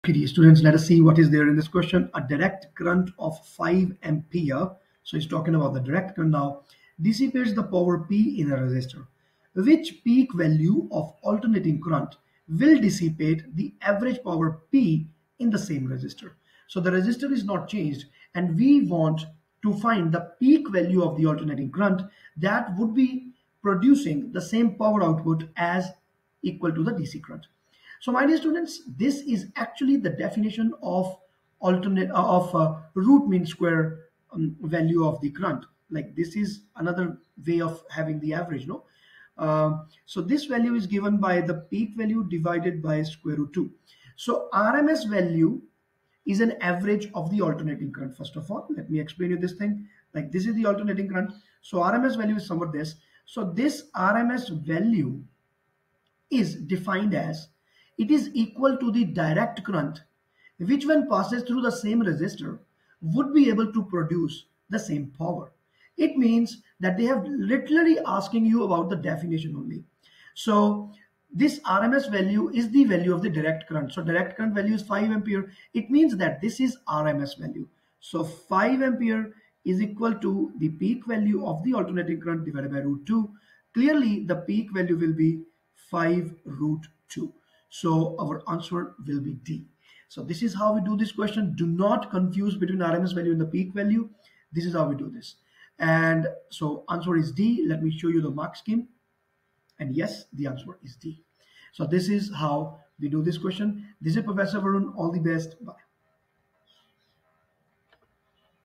A direct current (dc) of 5A dissipates a power P in a resistor. Which peak value of the alternating
A direct current (dc) of 5A dissipates a power P in a resistor. Which peak value of the alternating current (ac) will dissipate an average power P in the same resistor?